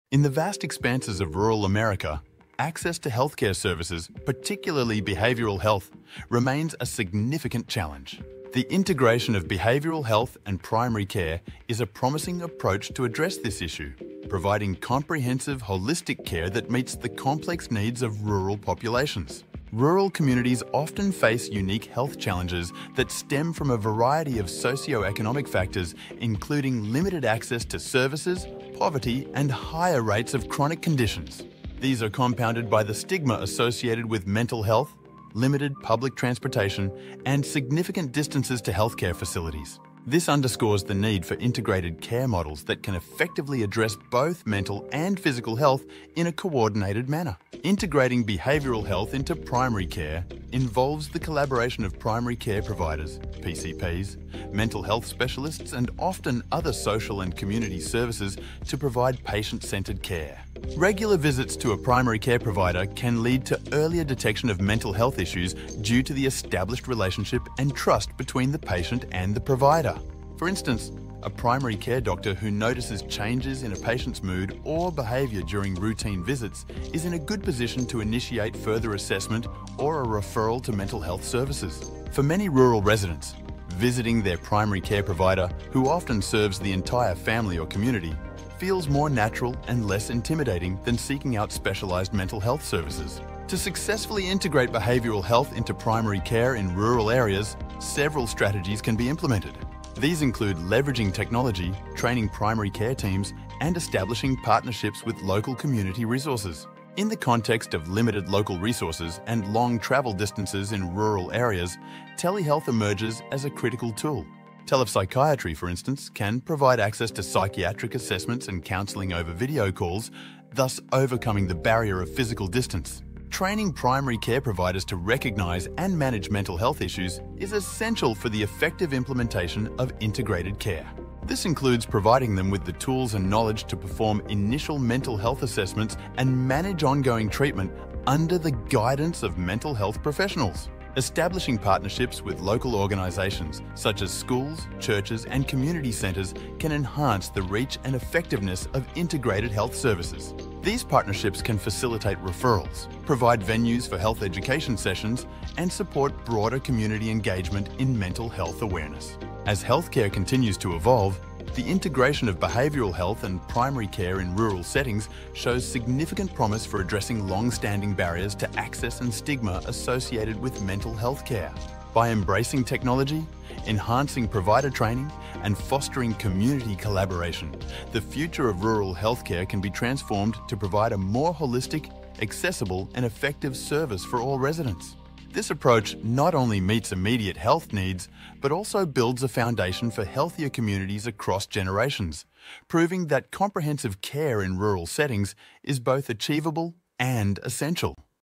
Lena Esmail | The Relationship Between Behavioral Health and Primary Care
🌾 Transforming Rural Healthcare: Integrating Behavioral Health into Primary Care 🌾

🎥 In rural America, accessing comprehensive healthcare, especially for behavioral health, is a profound challenge. This video dives deep into the innovative approach of integrating behavioral health with primary care, aiming to enhance the accessibility and continuity of care for rural populations. Discover how this holistic model not only addresses physical ailments but also mental health needs within these communities.

What You'll Learn:
Understanding Rural Healthcare Challenges: Explore the unique socio-economic factors that complicate healthcare delivery in rural areas, including limited access to services and the stigma around mental health.
Benefits of Integration: Learn how merging behavioral health with primary care can lead to early detection of mental health issues, reduce stigma, and improve overall health outcomes by treating the patient holistically.
Strategies for Success: Uncover effective strategies for implementing this integrated care model in rural settings, such as leveraging telehealth technologies, enhancing provider training, and building community partnerships.
Role of Leadership: See how leaders like Lena Esmail, CEO of QuickMed, champion these initiatives, advocating for telemedicine and integrated care to bridge the significant healthcare gaps in rural communities.
👨‍⚕️👩‍⚕️ Join us in understanding how strategic innovations in healthcare delivery can bring about significant improvements in the quality of life for rural populations. This model not only addresses immediate health challenges but also lays the groundwork for a healthier, more sustainable future.